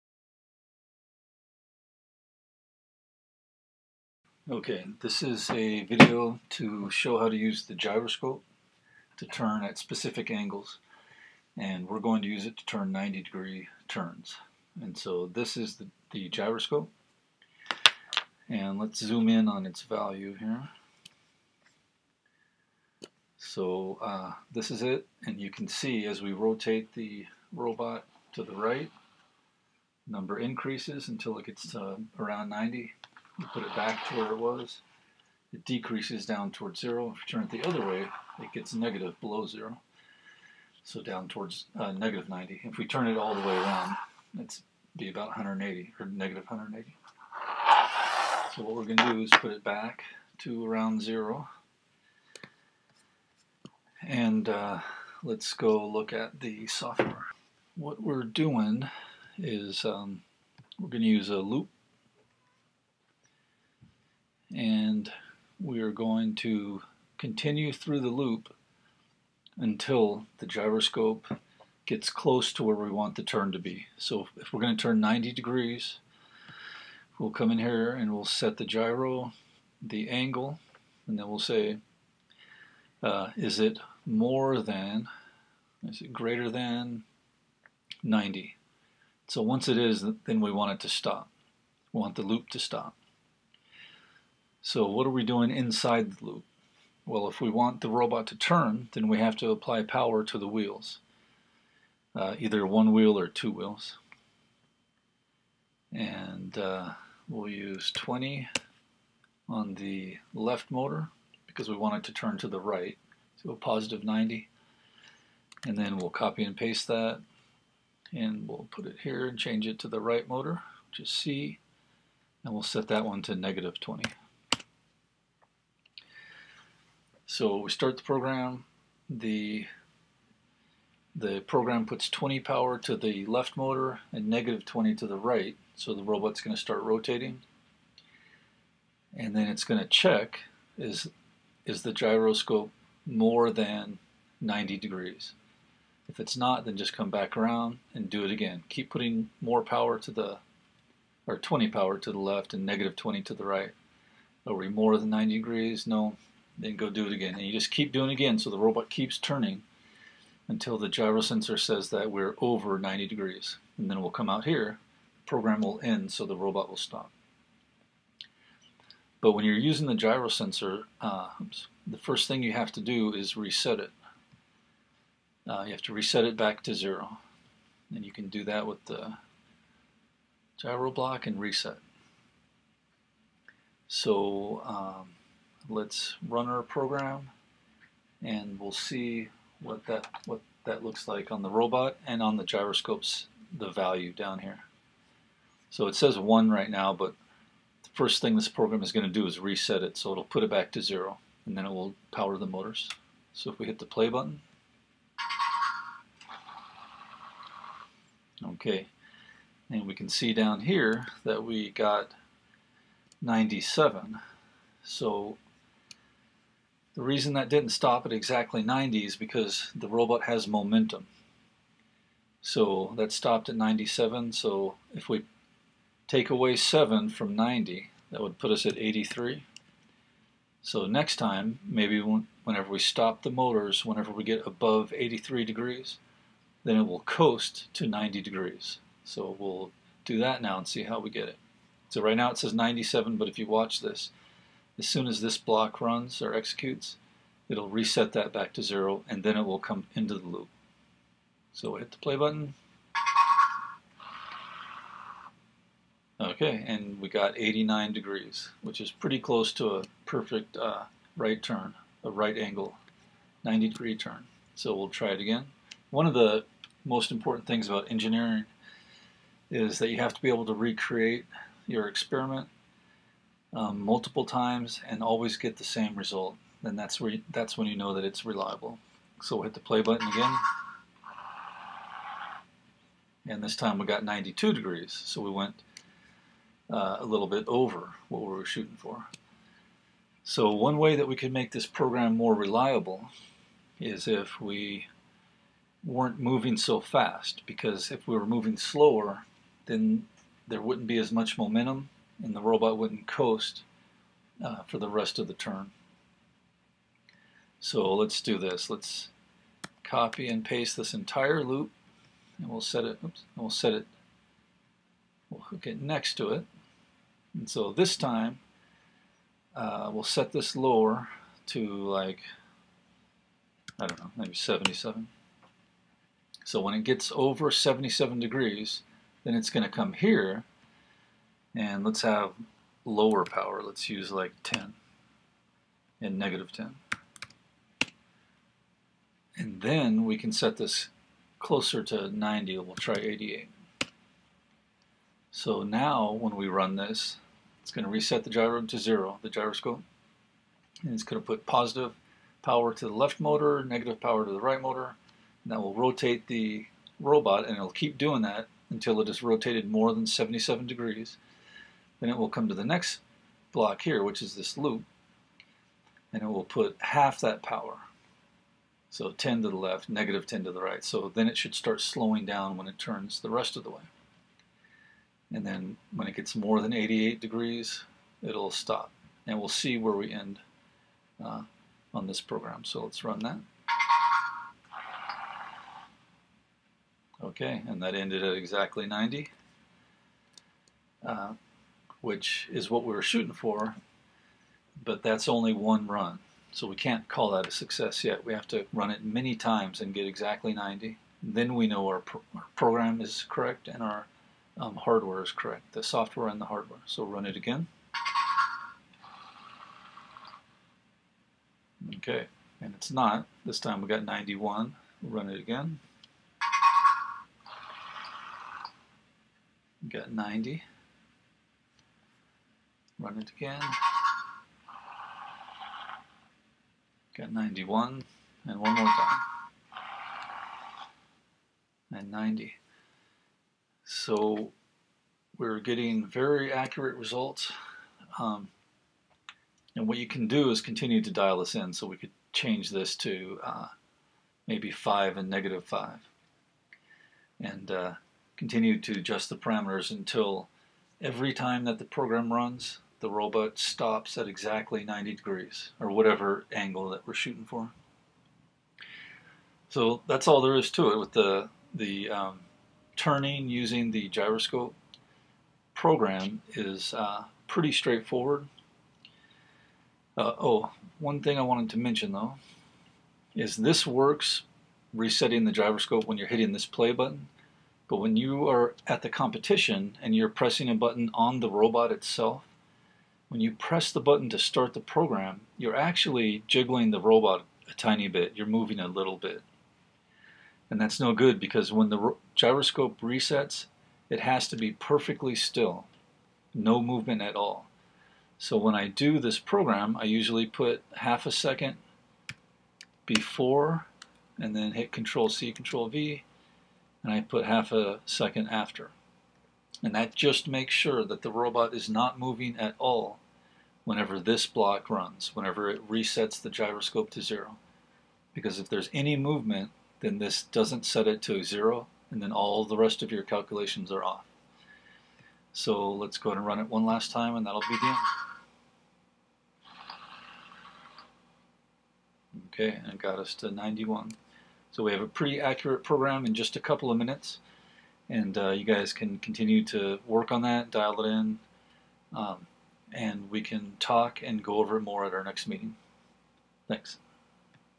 FLL Accurate turns using Gyroscope - Software
Lets go over how to make the EV3 turn a certain number of degrees every time.  Based on this we could also use the gyro to make the EV3 go perfectly straight forward.